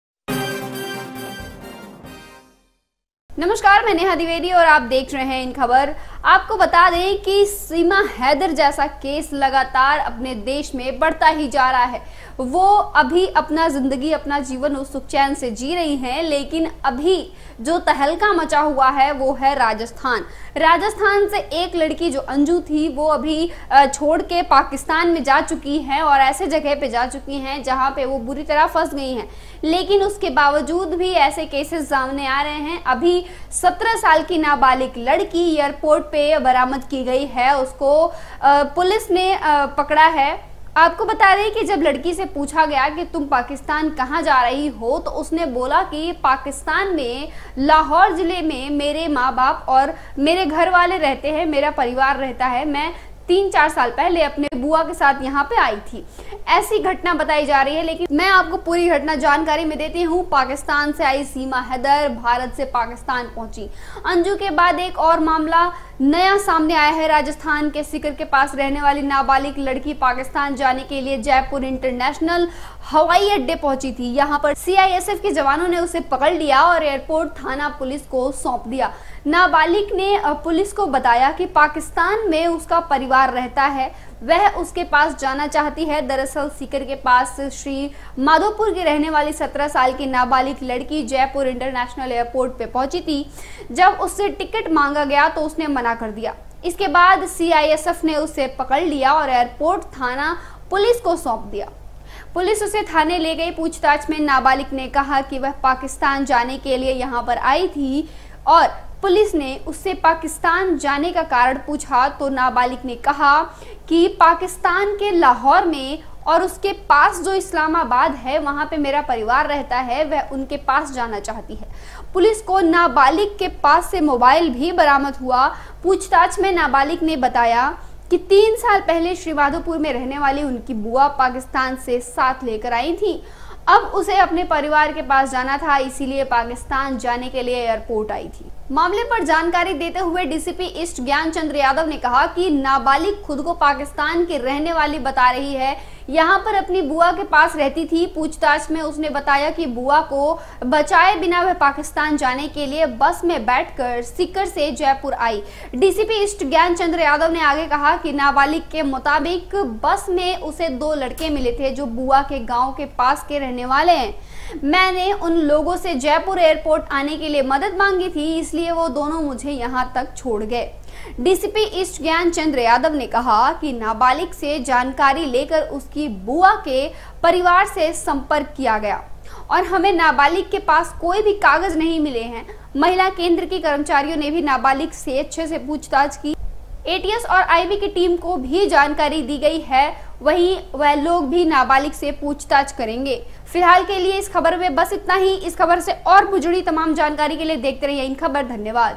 Jaipur Airport : जयपुर से पाकिस्तान जा रही थी लड़की, पुलिस ने एयरपोर्ट से पकड़ा पुलिस के हवाले
पाकिस्तान से आई सीमा हैदर, भारत से पाकिस्तान पहुंची अंजू के बाद एक और मामला सामने आया है. राजस्थान के सीकर के पास रहने वाली नाबालिग लड़की